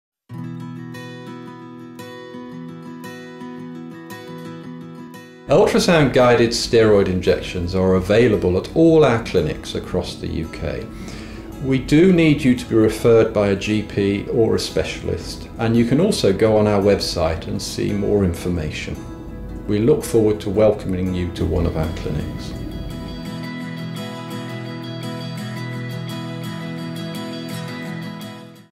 How Can I Get An Ultrasound Guided Steroid Injection?
Dr Paul-Zollinger-Read CBE, Chief Medical Officer for Medical Imaging Partnership shares how to be referred to a Medical Imaging Partnership centre for an Ultrasound-Guided Steroid Injection.

For more information on managing pain with Ultrasound-Guided Steroid Injections with Medical Imaging Partnership,